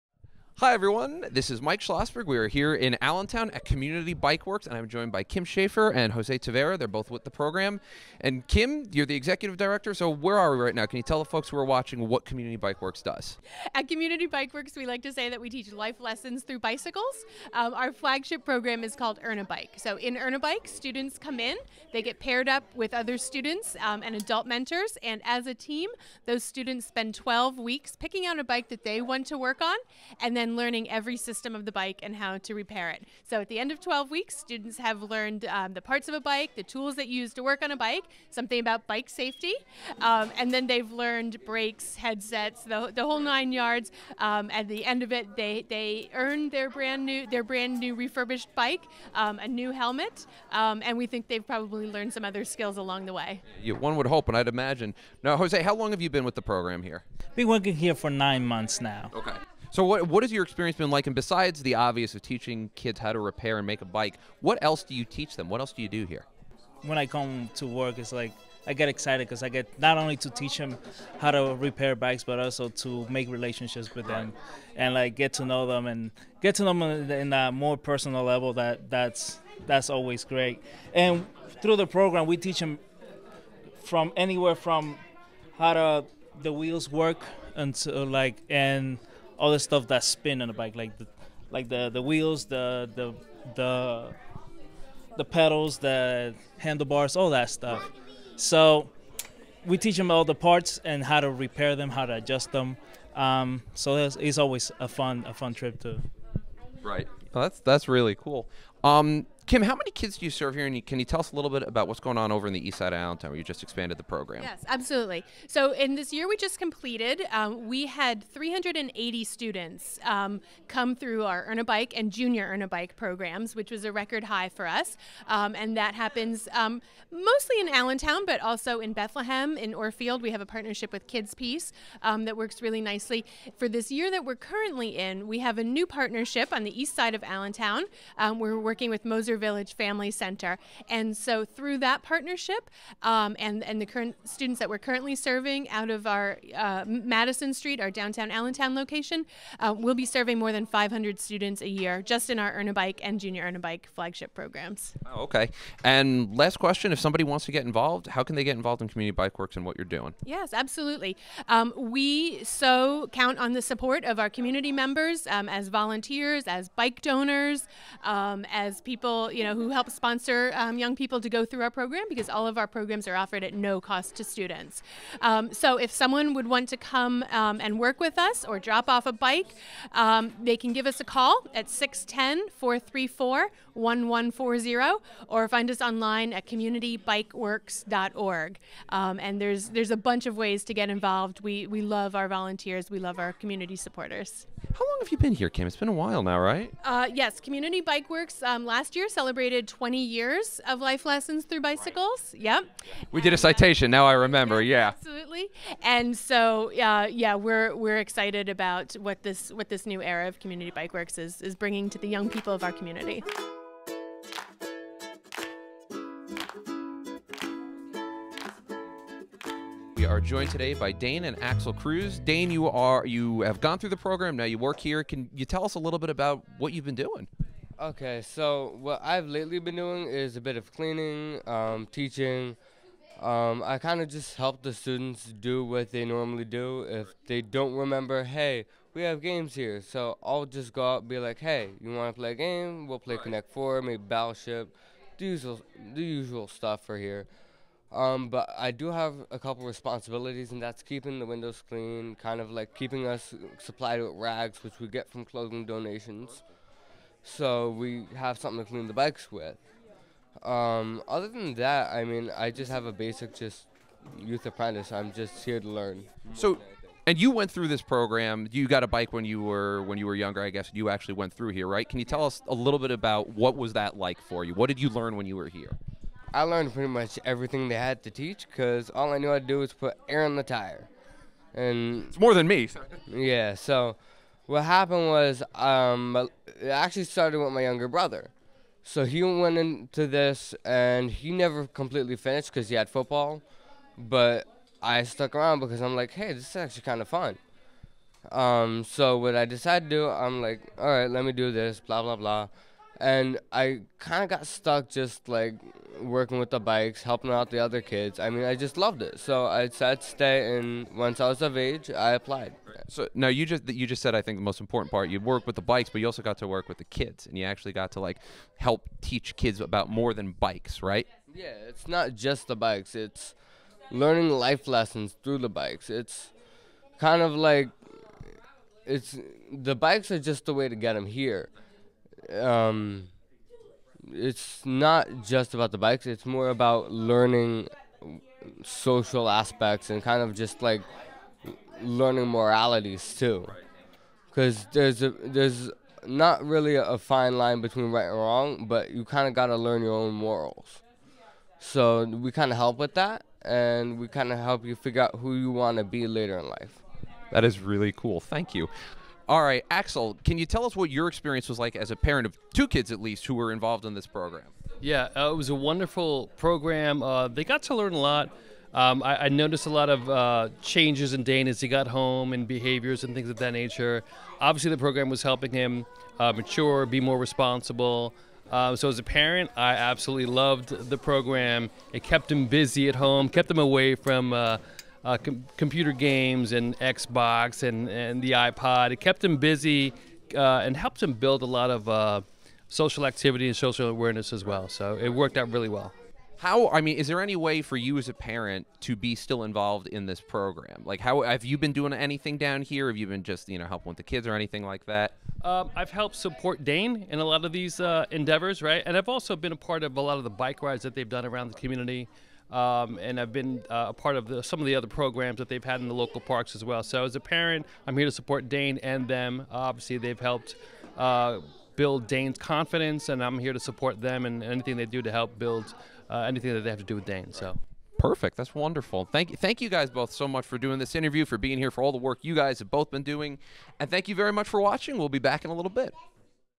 Schlossberg: Building Bikes, Building Character
Pa. state Rep. Mike Schlossberg visits Community Bike Works to learn how their flagship program "Earn a Bike" teaches students life lessons through the maintenance and repair of bicycles.